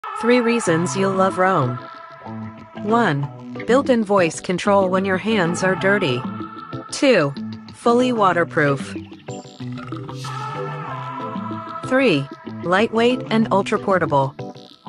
Backyard-ready with Roam
Lightweight, portable, and equipped to handle every splash and spray.

Discover Roam at sonos.com.